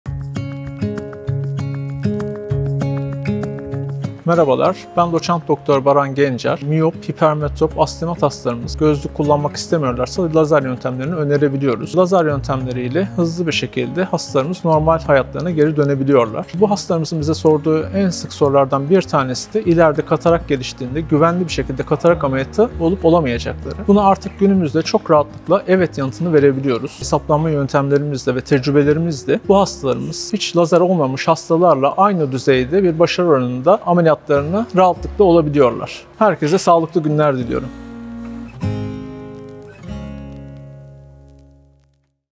Doç. Dr. Baran Gencer Lazer Tedavisi Katarakt Ameliyatına Engel Midir?
Çok merak edilen "Lazer Tedavisi Katarakt Ameliyatına Engel Midir? " sorusunu sizler için yanıtladı.

❕Sayfa içeriği bilgilendirme amaçlıdır.

Doktorumuzun Bilgileri İçin Aşağıdaki Linke Tıklayabilirsiniz.